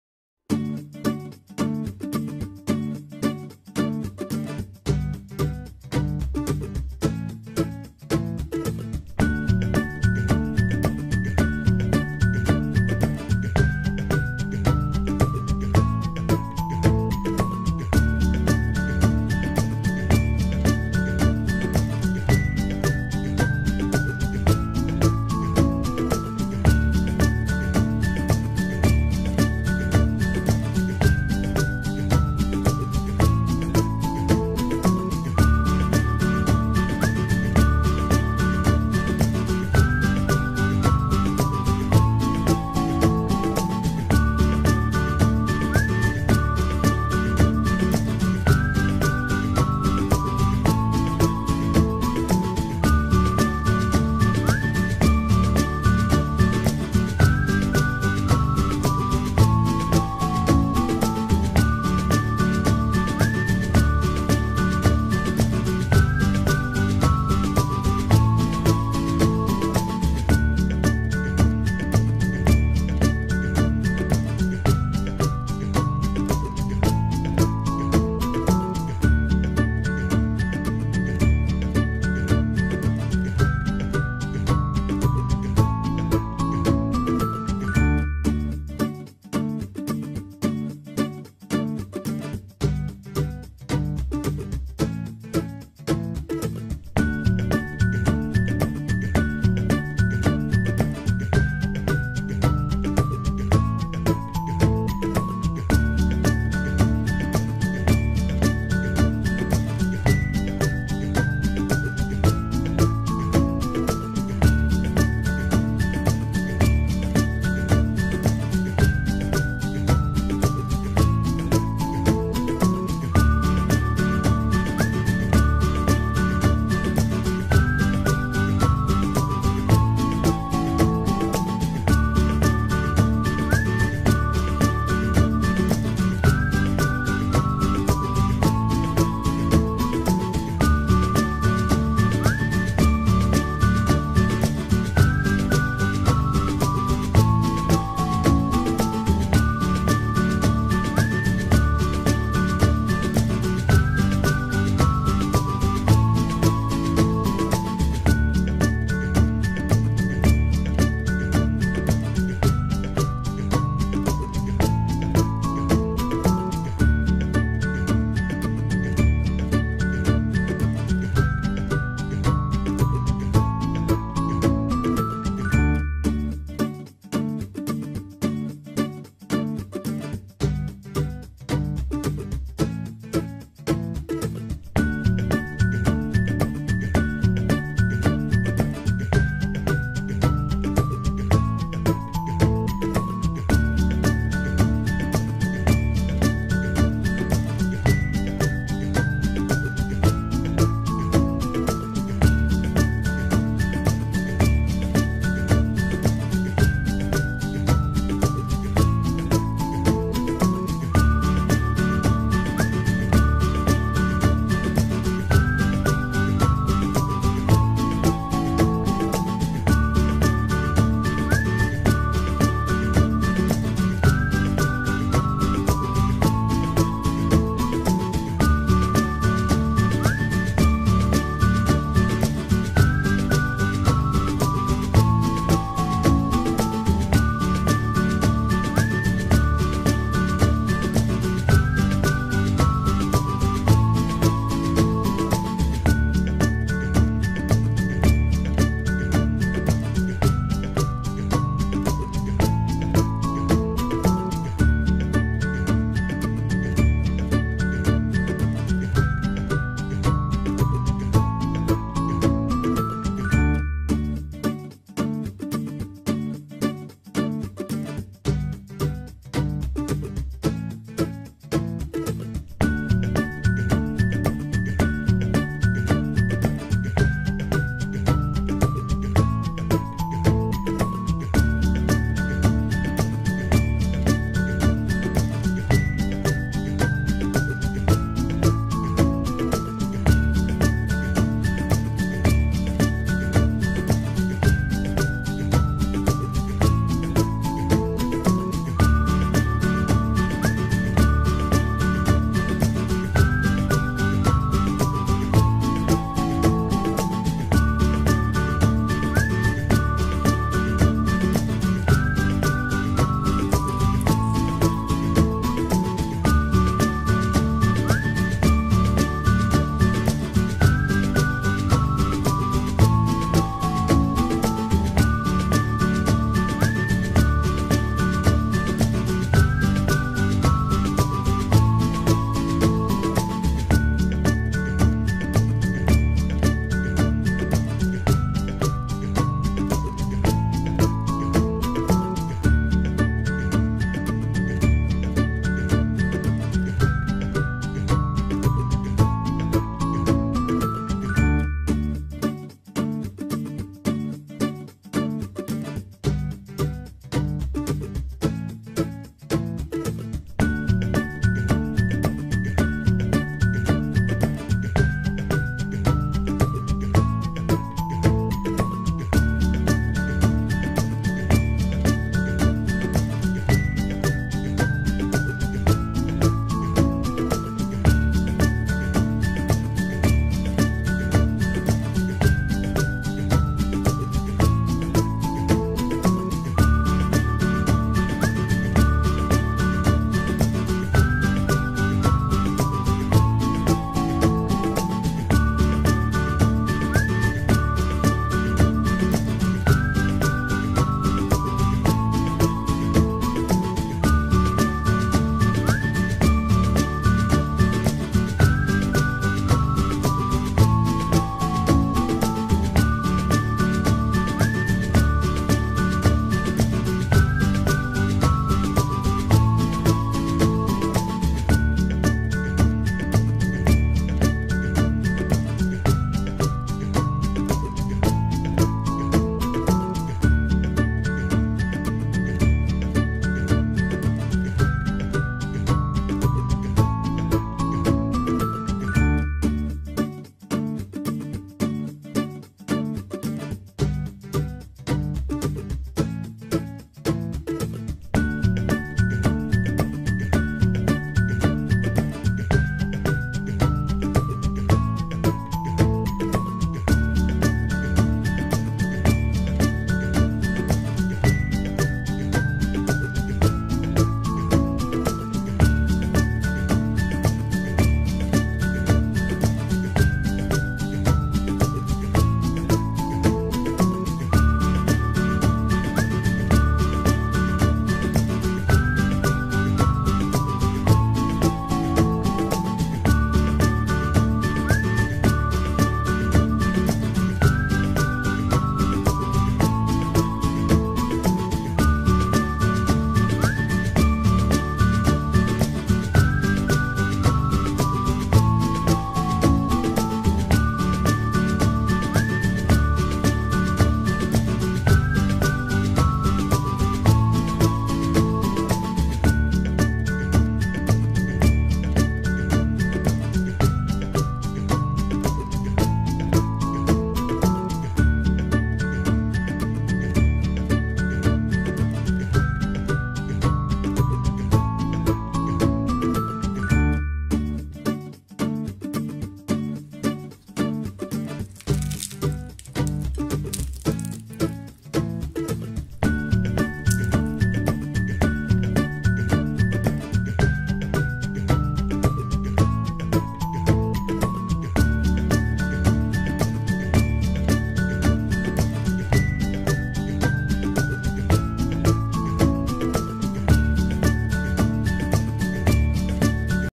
Khaja | Kaja | Divali Special | Divali Sweets | Mom's Version | Mauritius | TheTriosKitchen
Hello everyone! In this video, we will show you how to make Khaja easily at home. This delicacy is a must try for the upcoming Divali festival or on any special occasion . Hope you give this recipe a try & please do share your feedback with us.